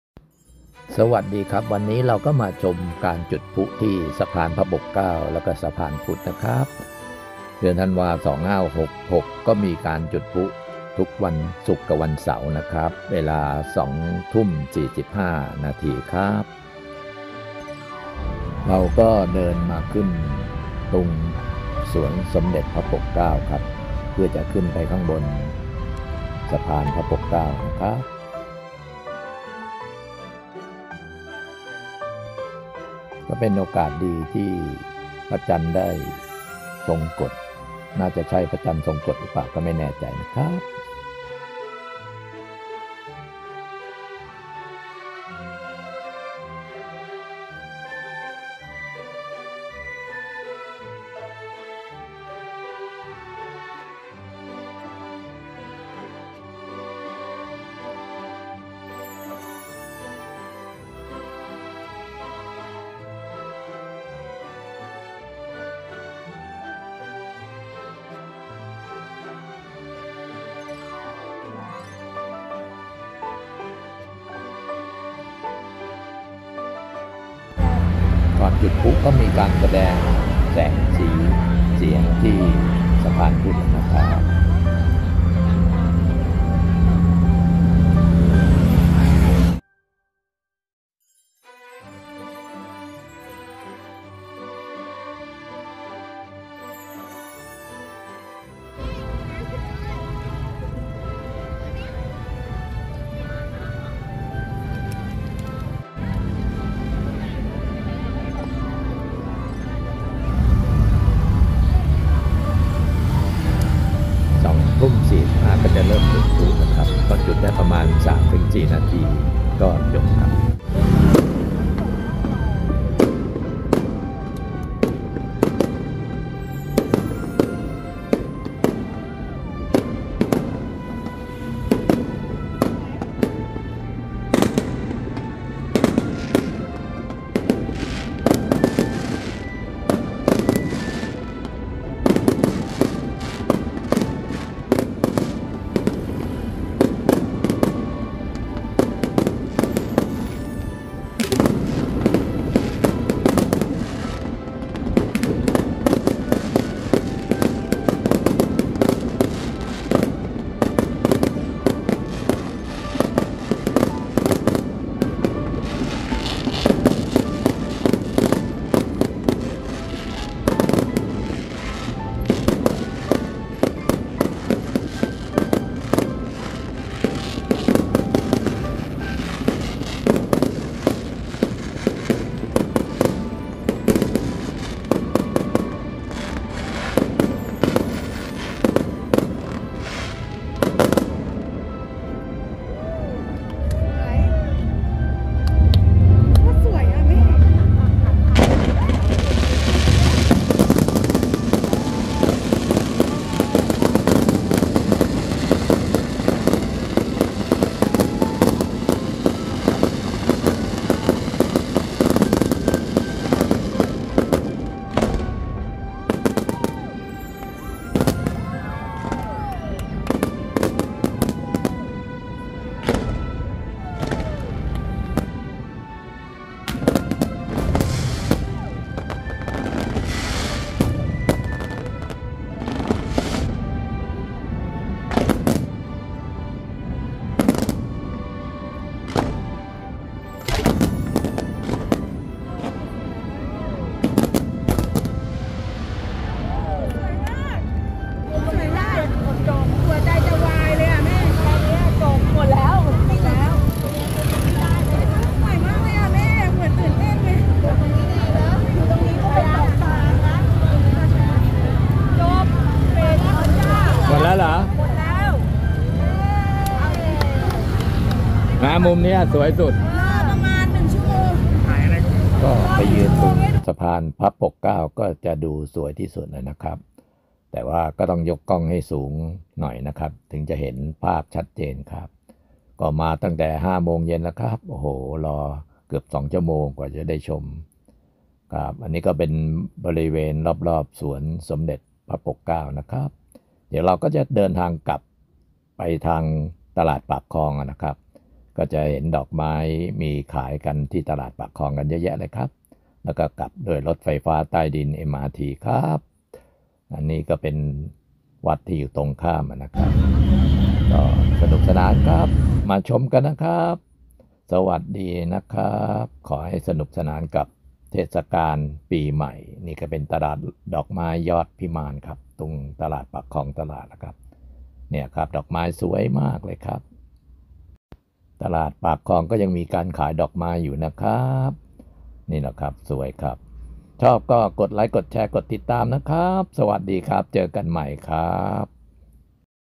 ชมพลุที่สะพานพระปกเกล้า
ตลอดเดือนธันวาคม 2566 มีกิจกรรมหลายอย่างตามลำน้ำเจ้าพระยา ทุกวันศุกร์เสาร์ก็มีการจุดพลุที่บริเวณสะพานพุทธและสะพานพระปกเกล้า ยืนดูบนสะพานพระปกเกล้าค่อนทางขึ้นฝั่งพระนครจะดูชัดและง่ายสุด